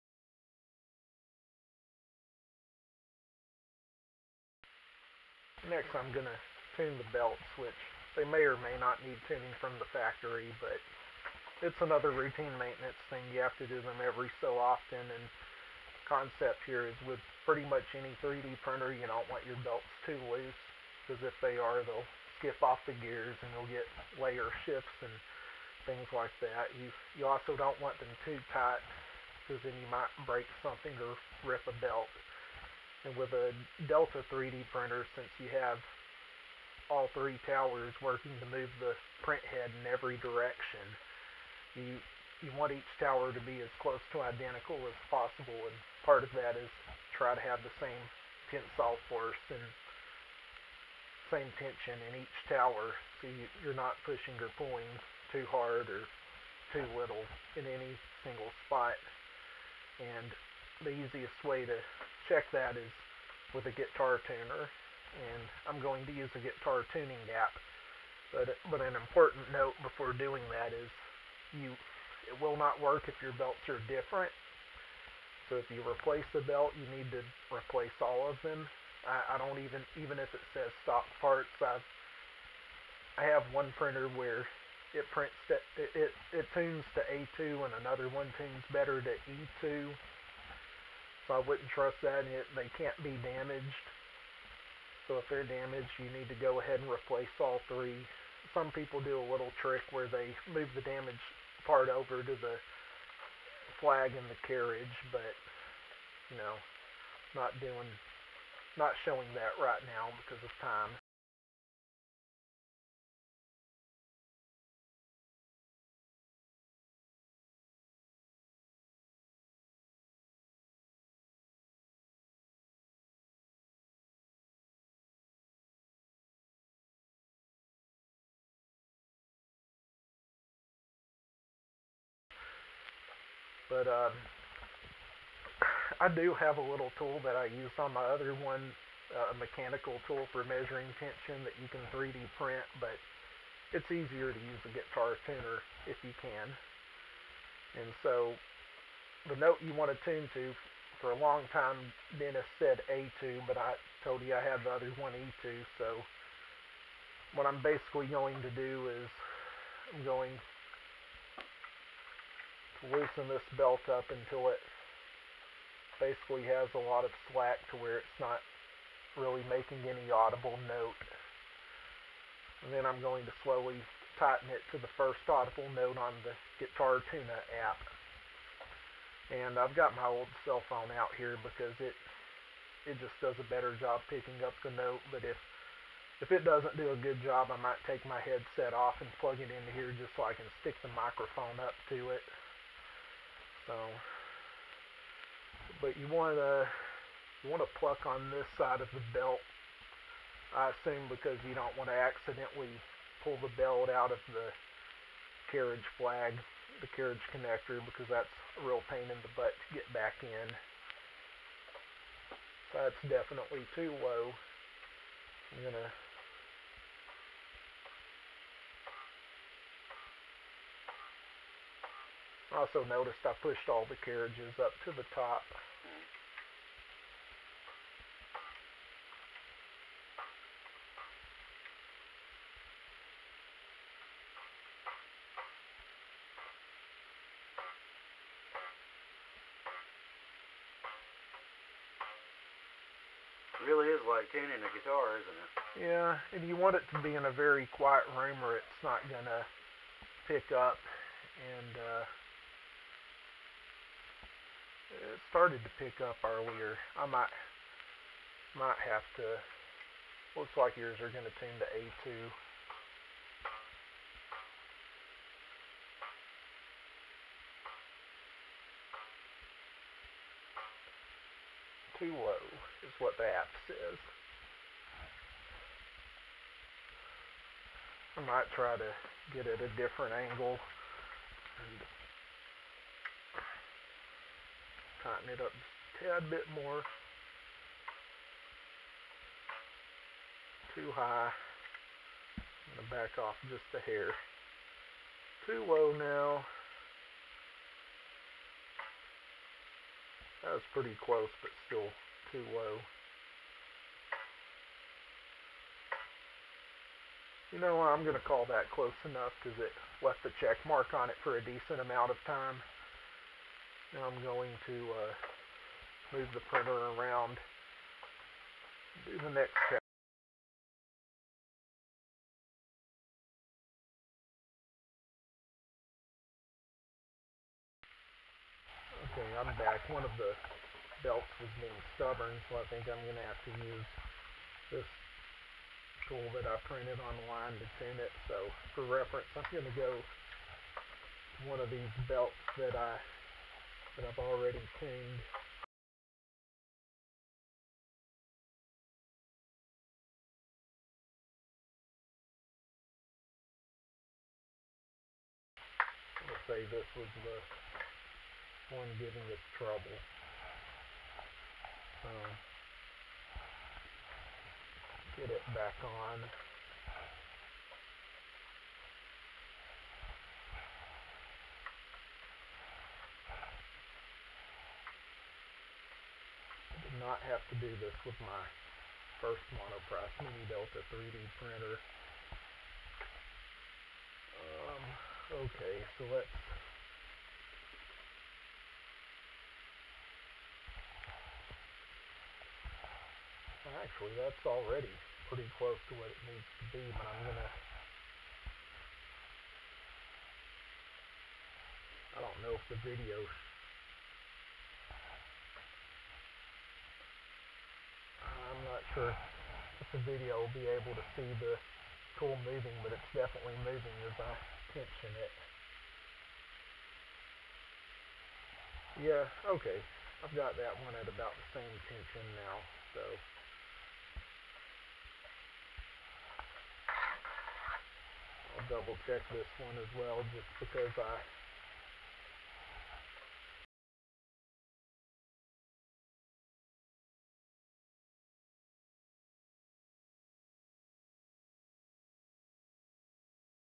MPMD Belt Tuning (with or without a guitar tuner) - I move my hand out of the way later, I promise.
Monoprice Mini Delta 3D Printer
READ THE VIDEO DESCRIPTION!!!
This is pretty much the same information contained in the existing calibration guides, except maybe there's more emphasis on the mechanical tool option.

I'm aware of the poor video quality and I don't care.

Explanation: 0:05
Guitar Tuner Method: 1:50
Belt Tension Meter Method: 5:28

Text Guide for tuning the belts.

Belt Tension Meter Reading Corresponding to Note A2.

 Misc MPMD Information

If you have questions, please go to either Reddit or Facebook (more active). YouTube is a crappy place to troubleshoot and you will find many other helpful people in those groups.